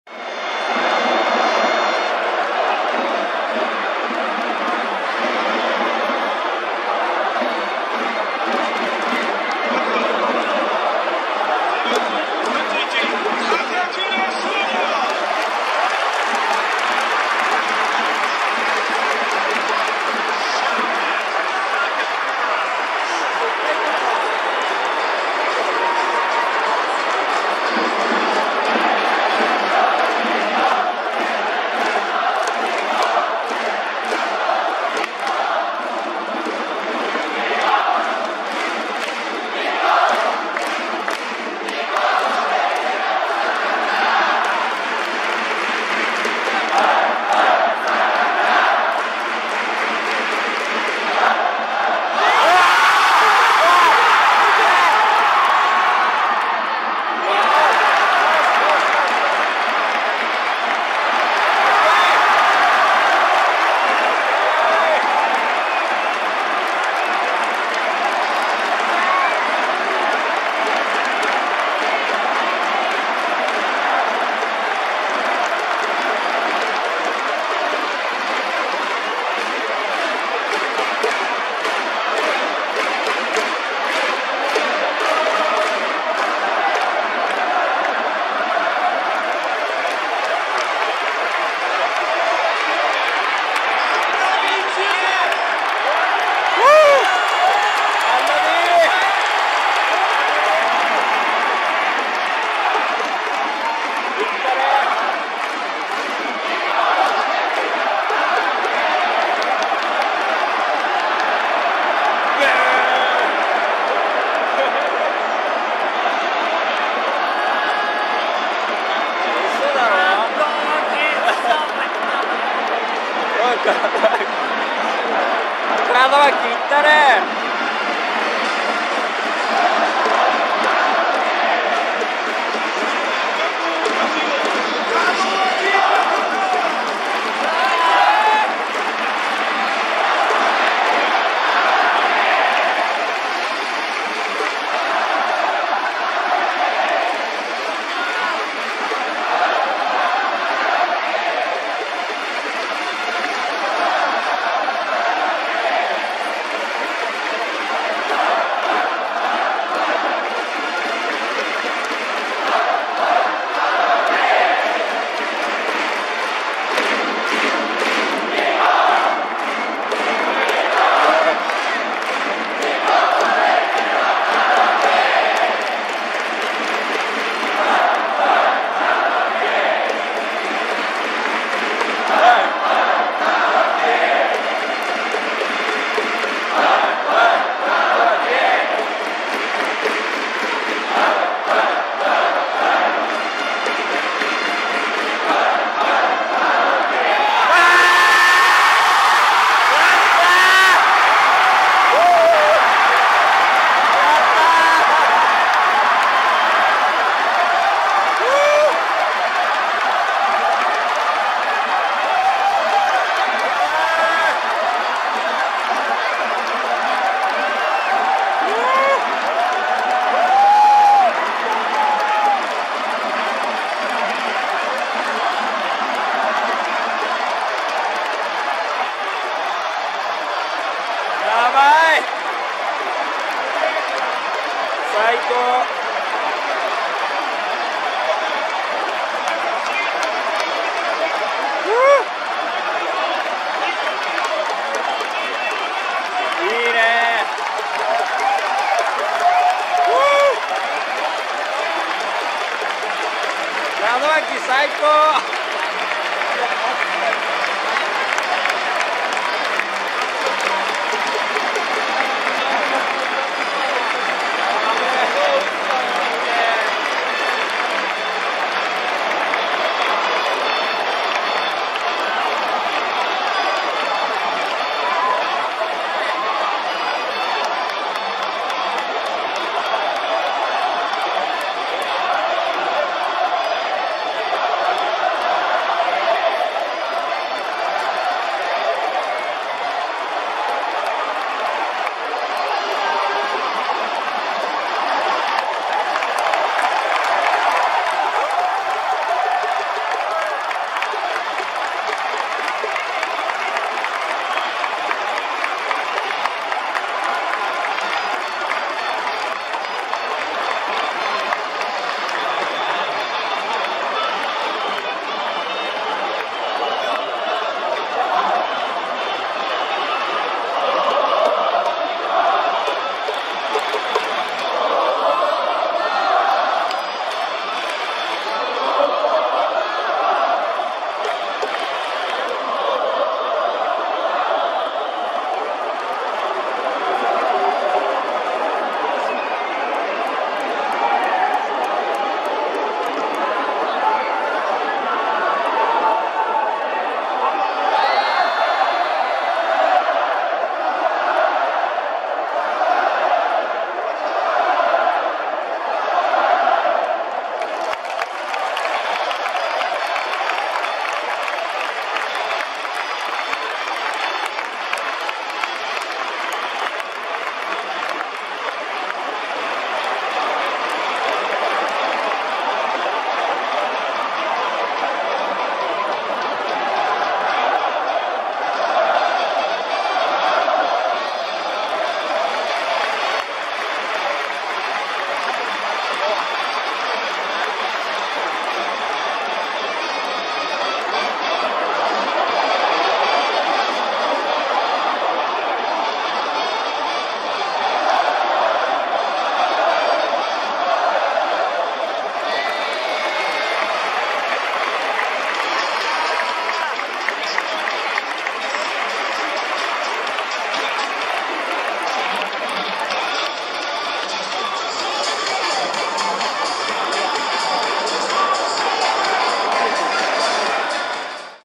侍ジャパン、門脇が日韓戦決勝で劇的サヨナラ打！優勝を決める瞬間がヤバすぎた！アジアプロ野球チャンピオンシップ2023 11/19(日)日本対韓国
エグい！門脇が日韓戦決勝で超劇的サヨナラ！侍ジャパンが優勝を決める瞬間！アジアプロ野球チャンピオンシップ2023 11/19(日)
日本対韓国　in東京ドーム